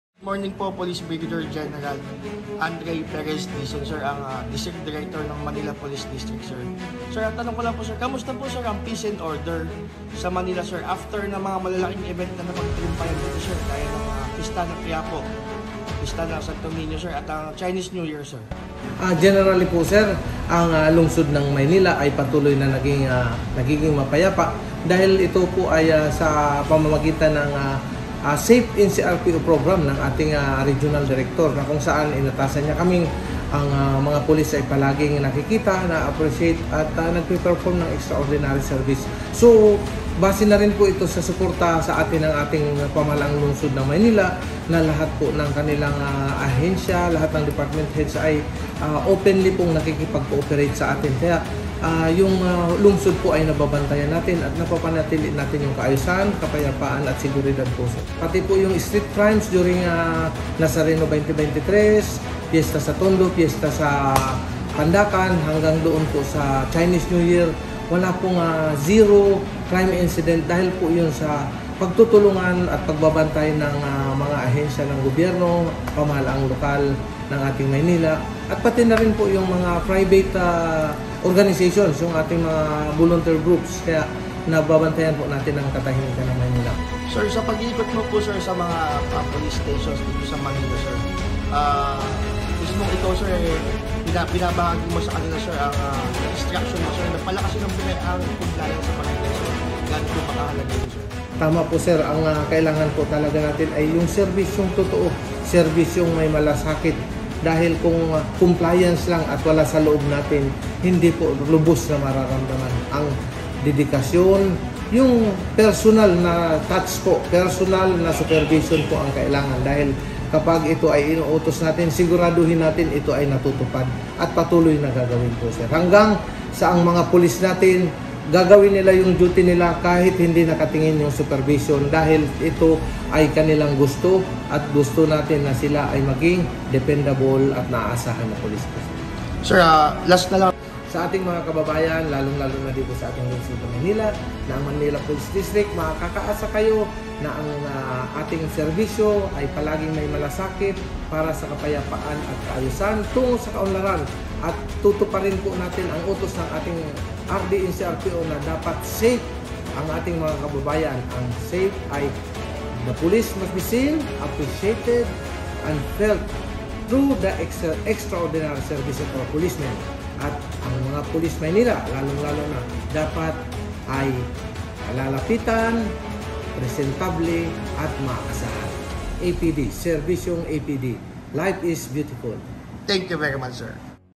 PEACE AND ORDER SA MAYNILA - PBGEN DIZON
INTERVIEW WITH MPD DIRECTOR PBGEN ANDRE PEREZ DIZON. re:Manila Peace and Order! Situation . Exclusive‼️
IBINAHAGI ni Manila Police District Director PBGEN ANDRE P DIZON na nakapagtala ng zero street crime incidents sa mga napagtagumpayan malalaking aktibidad ng MPD partikular na sa matagumpay at matiwasay na Nazareno 2023, Kapistahan ng Sto Nino sa Tondo at Pandacan at ang katatapos lamang na pagdiriwang ng Chinese New Year sa Binondo Maynila.

Napagtagumpayan aniya ito ng MPD dahil sa dedikdong na pagtupad ng tungkulin katuwang ang ibat-ibang ahensya ng Pamahalaan, Private at Volunteer groups at pakikiisa ng ating mga kababayan.

Dahil dito, Lubos ang Pasasalamat ni PBGen Dizon sa bawat isa partikular na sa maayos na pagsunod ng mga residente ng lungsod sa bawat alituntunin na inilatag sa mga nagdaang aktibidad.

Idinagdag pa ni Dizon na makakaasa aniya ang ating mga kababayan na sisikapin ng hanay ng MPD na mapaigting pa katahimikan at katuwasayan (peace and order) sa lungsod para sa kapakinabangan ng bawat isa kasabay ng kanyang panawagan na huwag magatubiling magsumbong o magbigay ng impormasyon sa social media ng "Manila Police District" laban sa kriminalidad at droga na tiyak na aalsyunan ng mga Pulis Maynila. Alinsunod sa programag SAFE NCRPO ni PMGen Jonnel Estomo, RD NCRPO.

Bilang AMA ng kapulisan ng MPD ay hindi aniya titigil si PBGEN DIZON na gabayan at magsagawa ng spot inspection and correction sa ibat-ibang prisinto upang masiguro ang maayos na pagsunod sa polisiya at alituntunin sa tapat na pagpapatupad ng tungkulin para sa kapakinabangan ng bawat isa.

Mula nang manungkulan bilang District Director ng MPD si General Dizon ay walang tigil ang pagiikot nito sa mga prisinto kung saan tatlong beses sa isang araw ang naging best practice na inspeksyon nito umpisa sa madaling araw, bisikleta naman sa umaga at motorsiklo sa gabi upang  maiyak naman ang "Genuine Police Visibility" na latag ng pulisya sa lansangan.

"Serbisyong palaging may MALASAKIT!"
Kahanga-hangang lider ng MPD! Kudos sir!
(MPD IS IN GOOD HANDS!)👍🏽☝🏼👍🏽
Approachable Presentable Dependable!